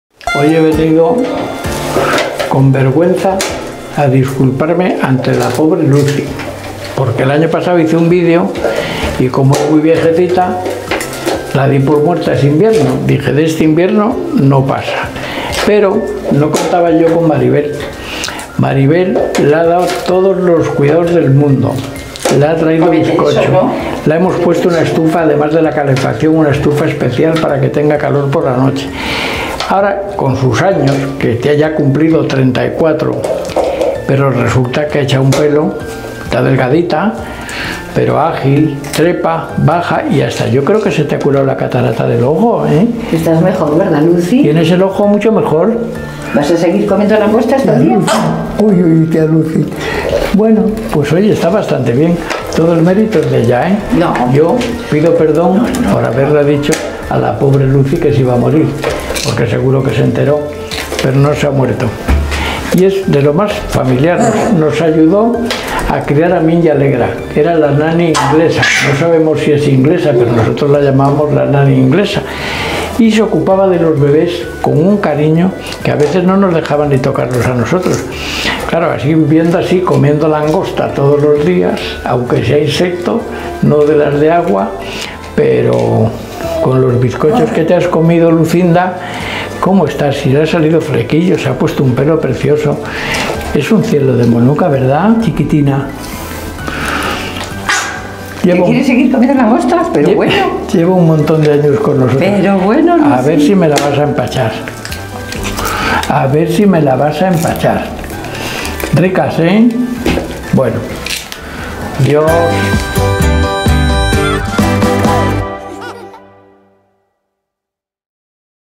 Así mima Maribel a Luzie
Hoy, José Ignacio Pardo de Santayana nos habla de la Tía Luzie y de lo bien que está a sus 34 años con los cuidados que le da Maribel en el ZoodeSantillana.
¡No te pierdas el vídeo de hoy y ventealzoodesantillana!